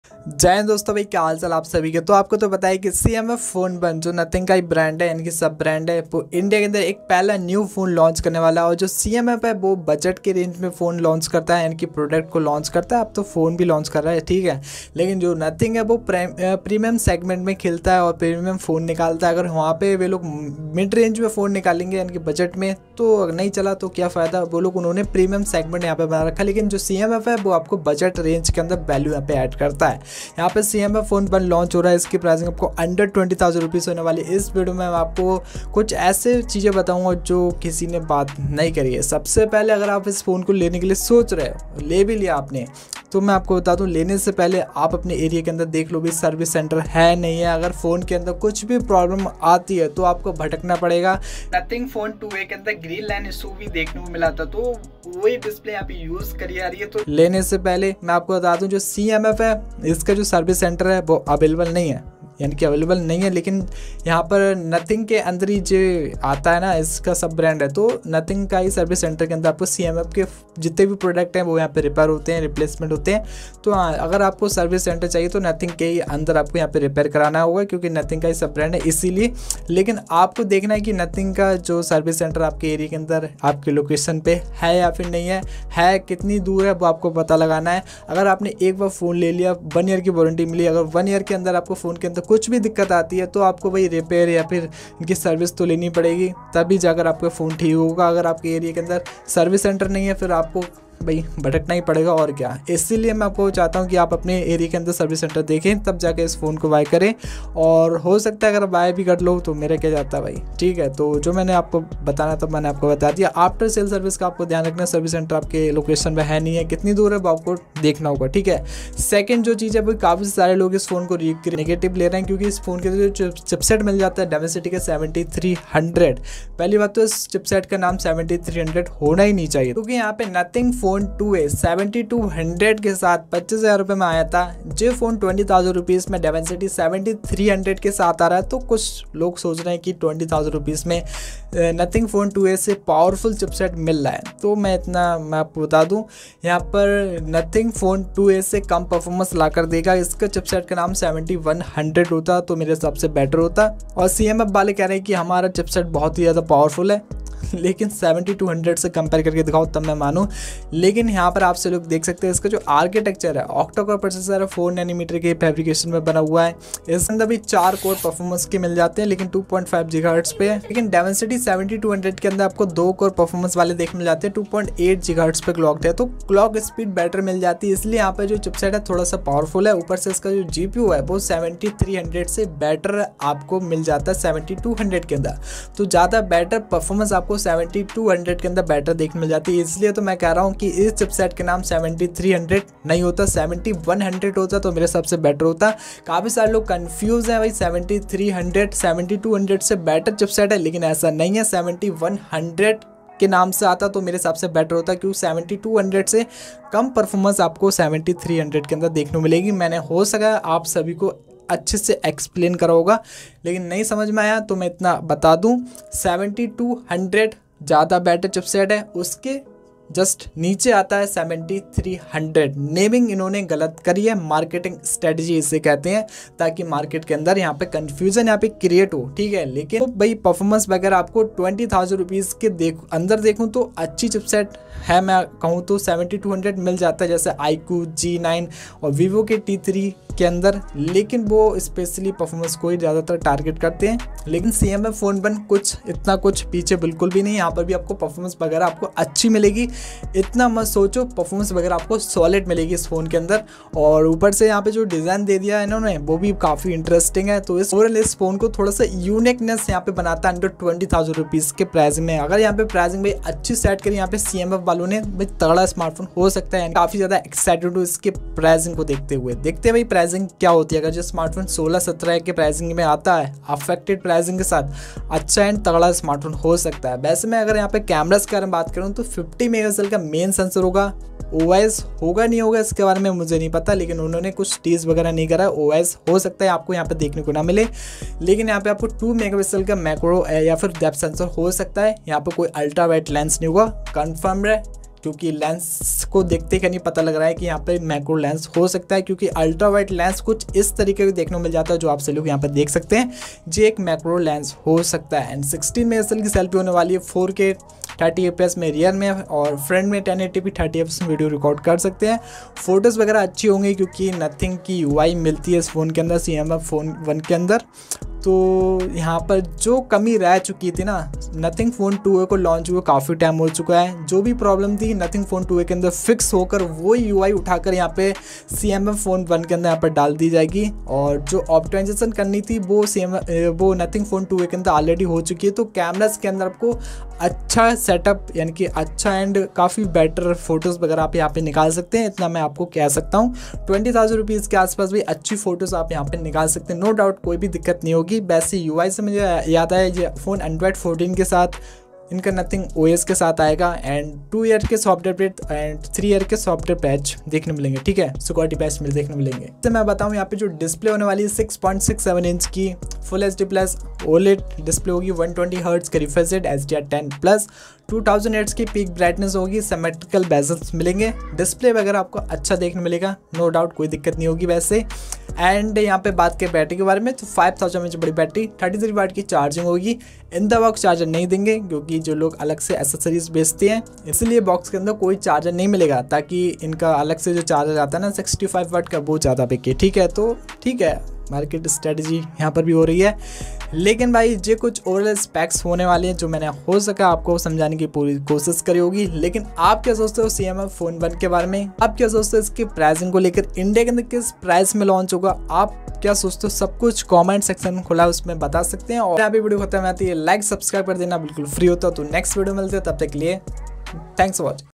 CMF Phone 1 - Don’t Buy Before Watching this Video, CMF PHONE 1 UNBOXING & REVIEW, india price
Hello Guys Aj Ki Iss Video Mei Meine Apko cmf phone 1 ka confirm specifications & features or india price ka Bara me bataya hai